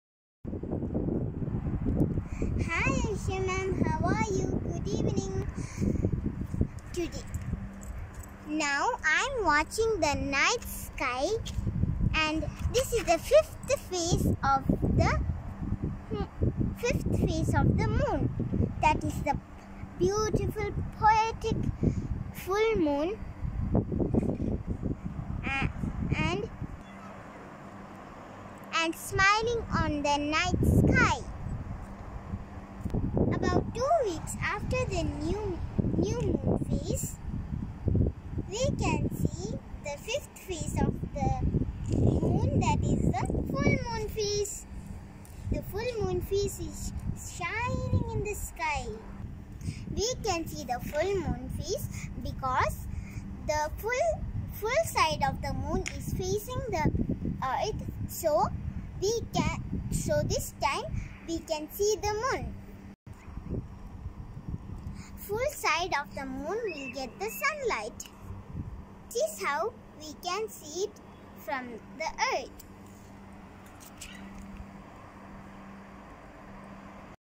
Full Moon phase of Moon. Nehal Fathima Grade 2
Video from Al Mudhaibi Modern Private School. Sinaw